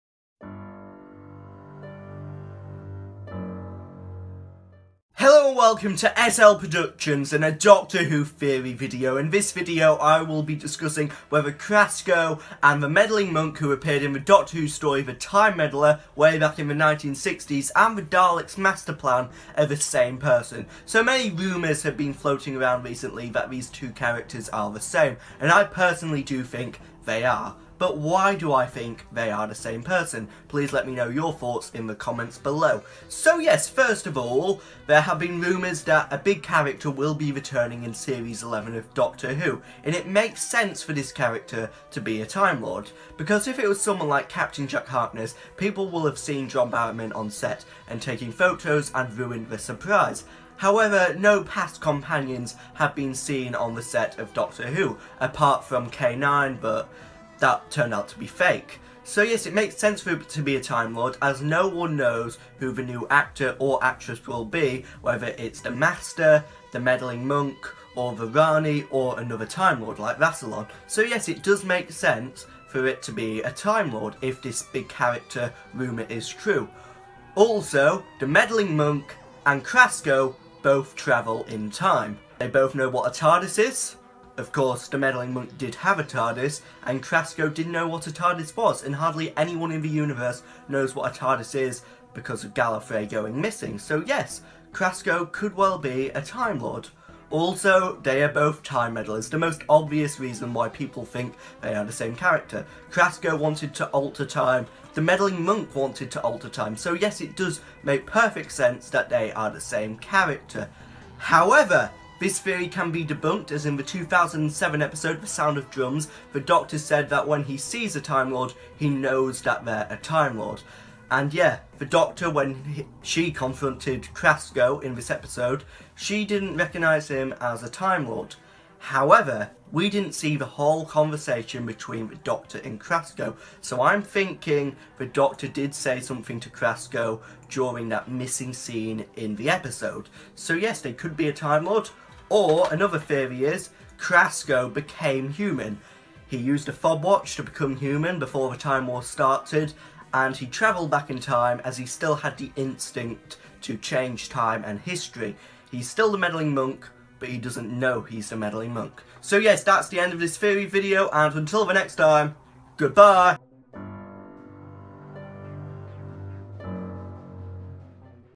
Are Krasko And The Meddling Monk The Same Person? - Doctor Who Theory
Is Krasko from Rosa and The Meddling Monk from The time Meddler the same person?